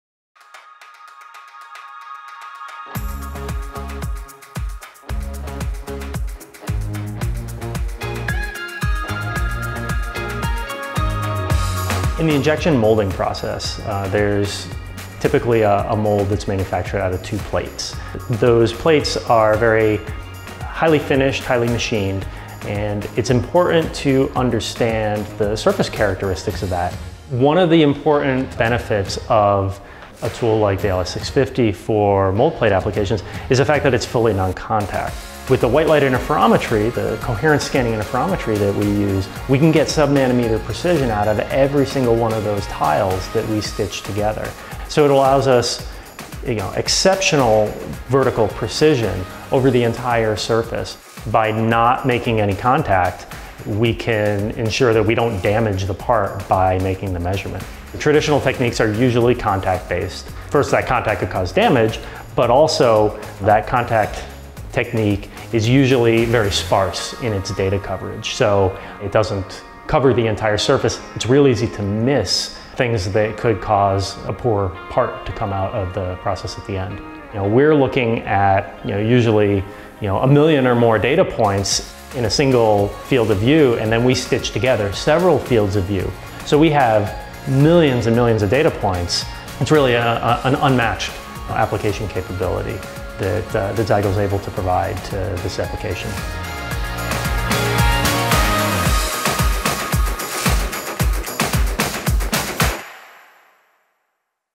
Nexview™ 650 3D Surface Profiling System - Mold Plate Application
The Nexview™ 650 metrology system is an inspection tool for automated measurement of injection molding tooling, PCBs, glass panels and other samples requiring an extended work volume up to 650 x 650 mm. It provides 2D & 3D measurements of a variety of surface features with sub-nanometer vertical precision and sub-micron lateral precision.